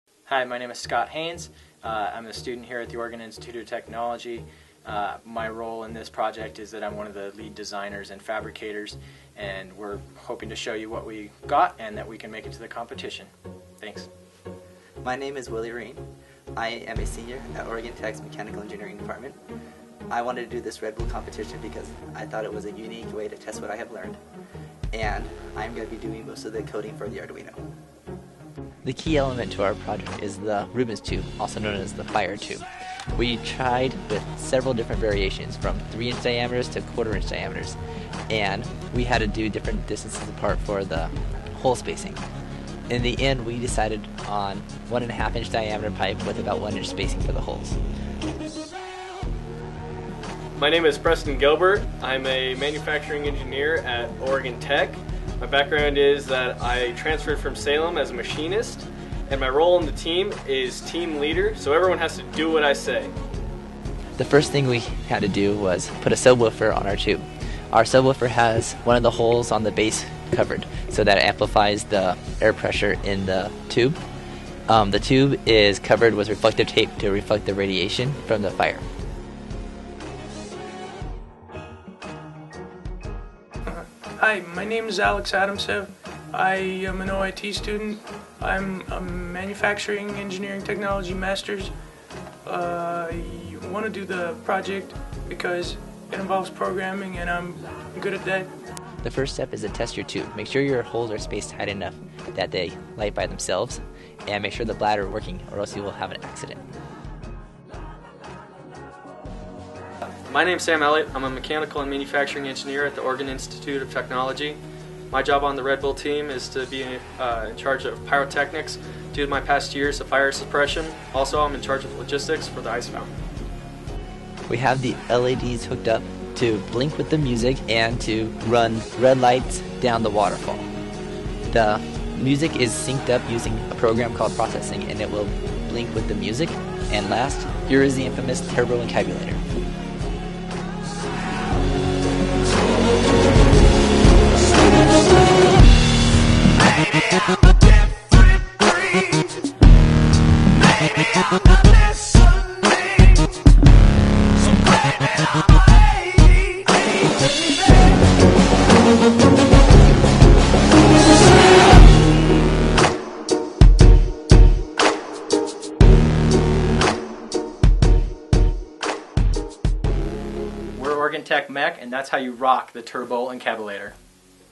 Oregon Tech Mech- Red Bull Creation 2013 Qualifier Entry
Oregon Tech Mech's entry into the 2013 Red Bull Creation contest.